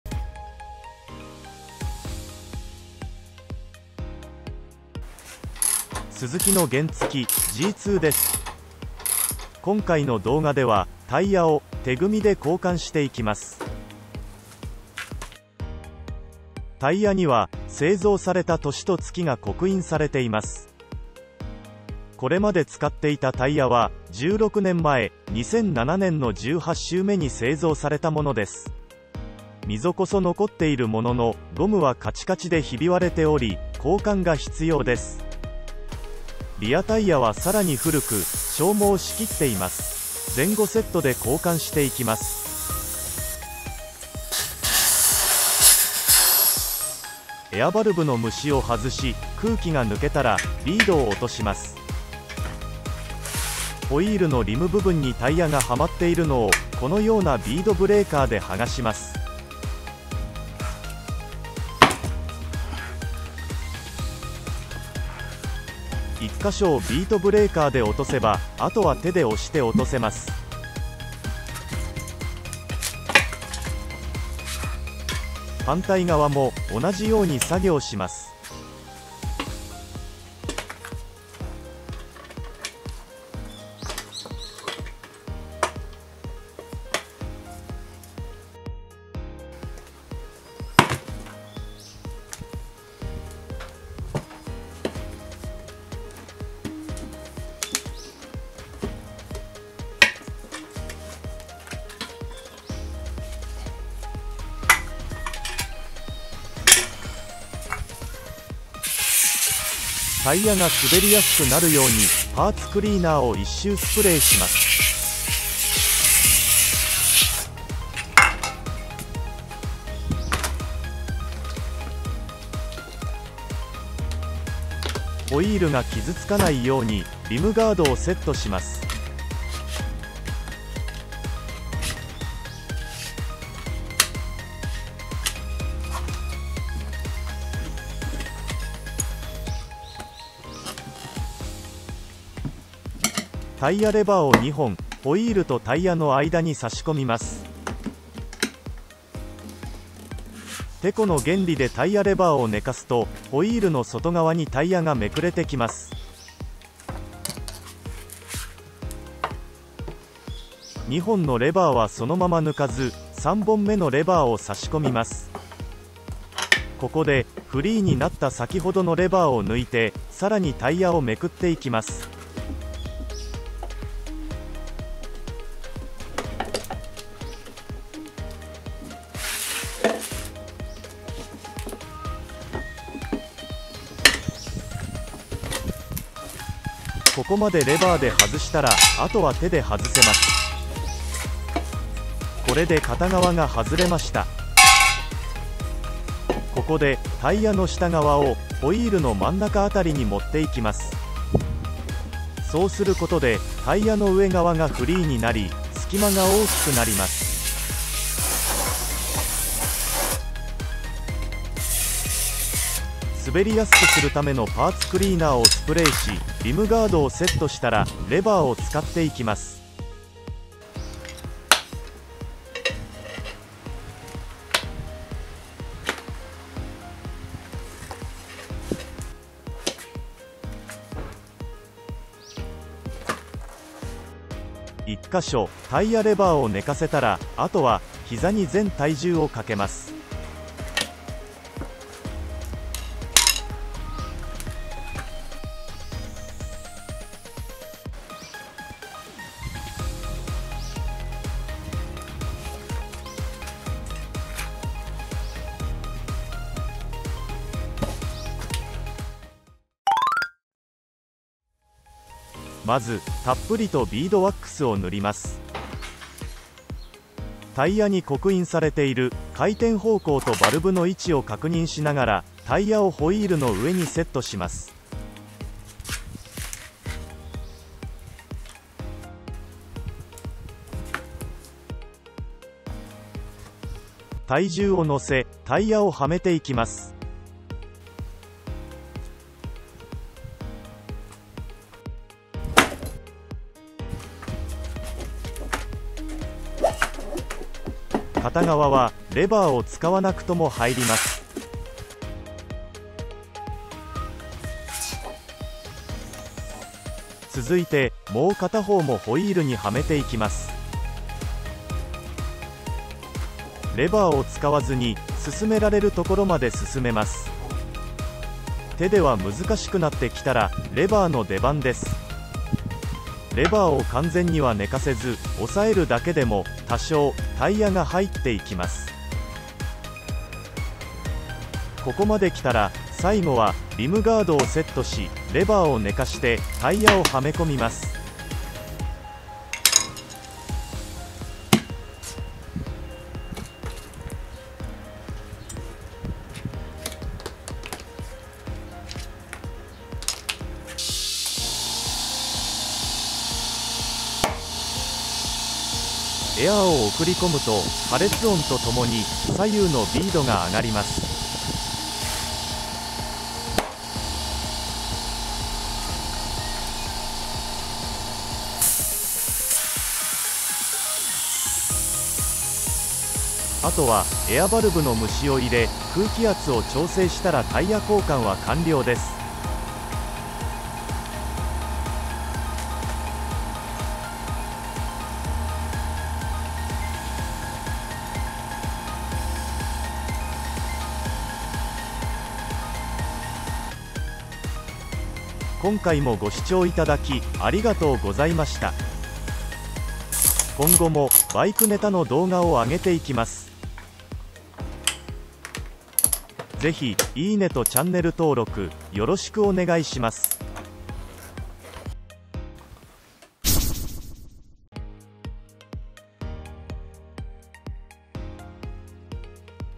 原付スクーター手組みDIYでタイヤ交換
スズキ原付スクーターZZ(CA1PB)をハイグリップタイヤTT93GPに交換しました。
フロント　110/70-12
リア　120/70-12